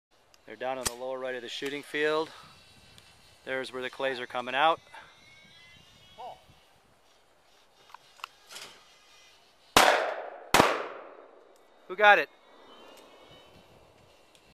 Al Longtin & Rob Weiss Shooting Clays
Al Longtin & Rob Weiss Shooting Clays in Breckenridge